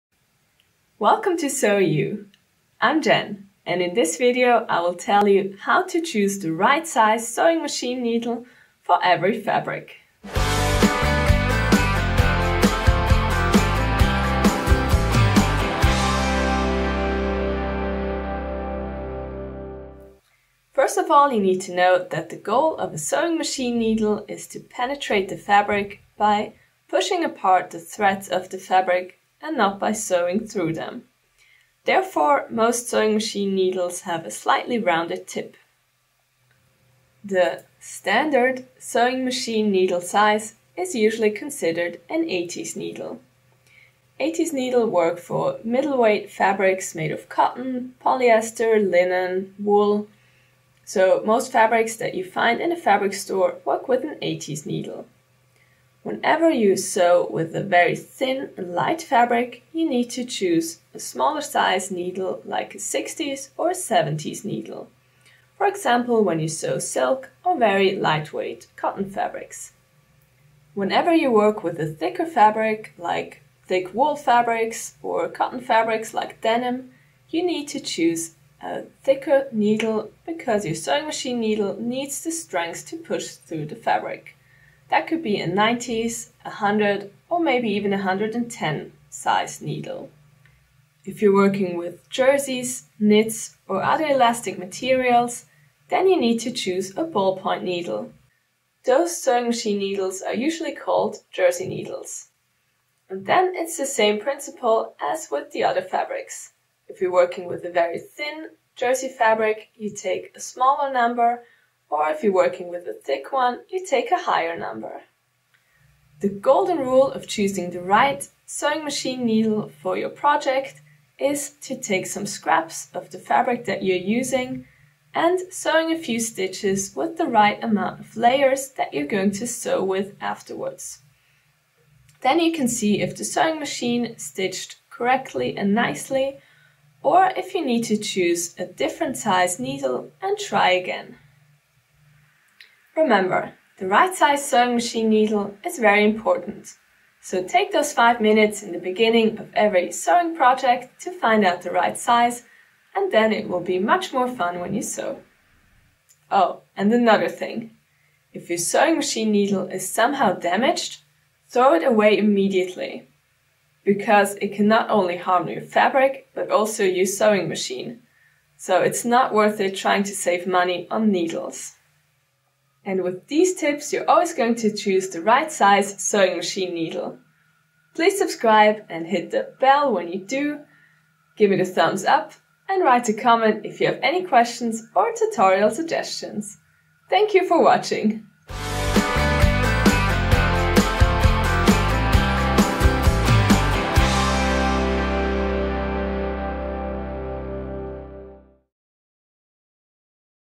Which needle size for what fabric
How do you know when to use which sewing machine needle size? Find out how to choose the right sewing machine needle size for every fabric.

I always upload the same video in English and in German.